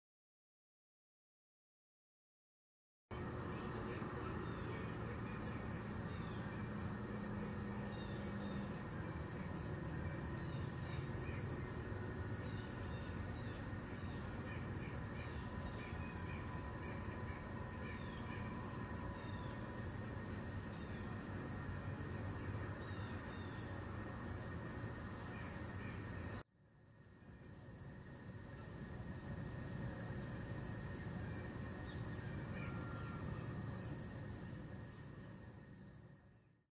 Beautiful Ruby Throated female hummingbird feeding on Salvia flower
Female Ruby Throated Hummingbird feeding on red, white and purple salvia flower in NW NJ 08 20 16 shot with Canon Powershot SX520HS